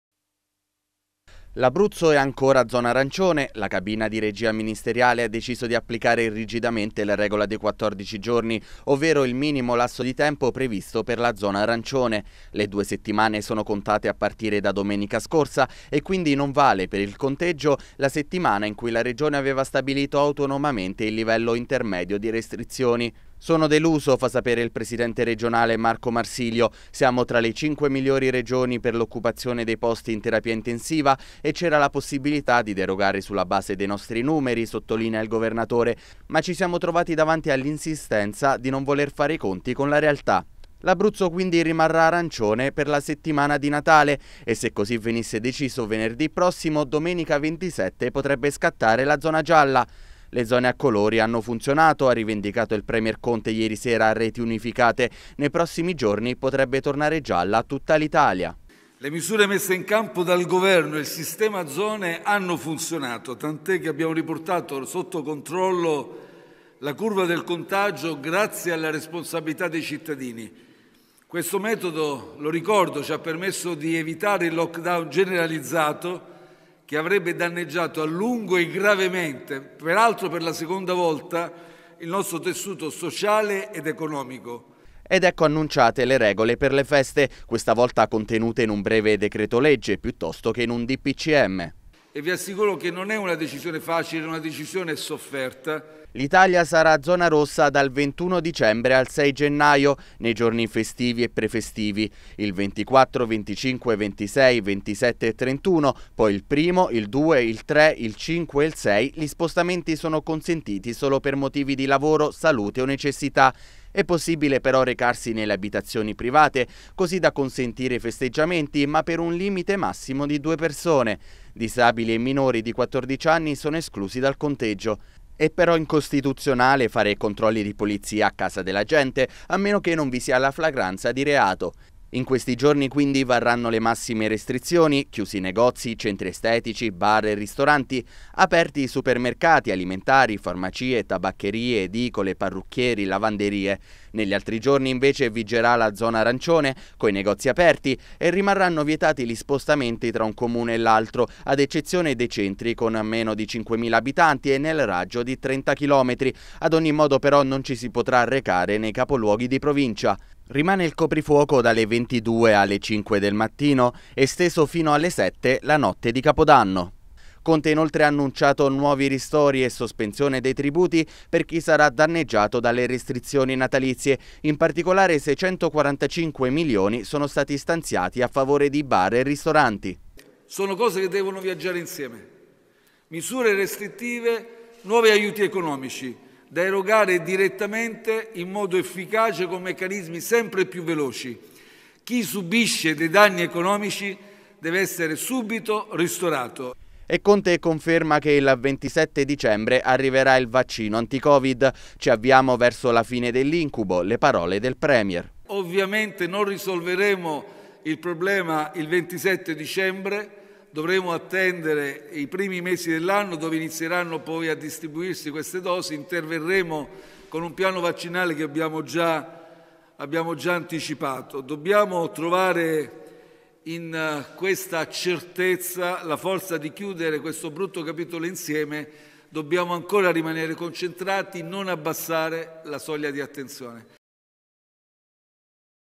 L'Abruzzo rimane arancione, ecco le regole per Natale e Capodanno
Ieri sera il premier Conte ha presentato il decreto Natale, con tutte le restrizioni previste a ridosso delle feste. Conte promette nuovi ristori e conferma l'arrivo del vaccino entro la fine dell'anno. L'Abruzzo intanto non ha ricevuto il declassamento a zona gialla, deluso il governatore Marsilio.